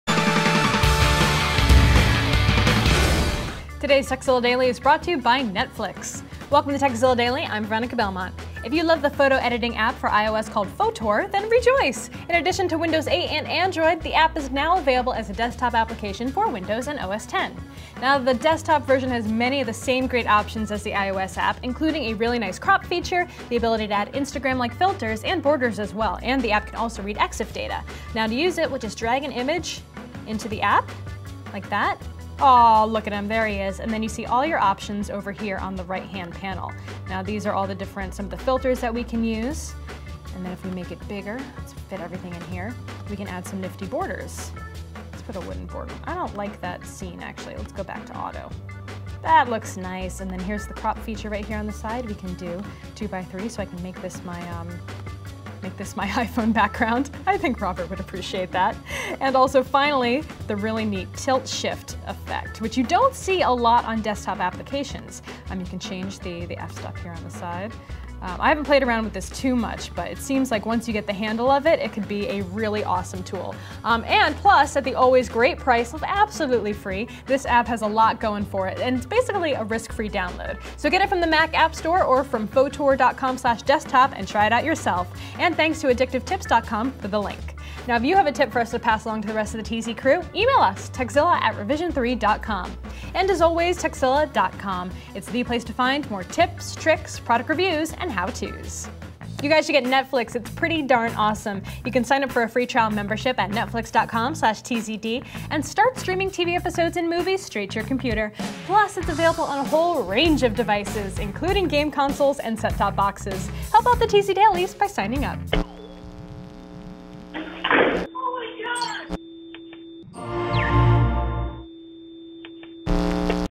Great Free Photo Editor for Mac and Windows
Fotor is a great photo editing application that started on iOS and is now available for Android, Mac, and Windows. Make simple lighting adjustments, add frames, filters, and more!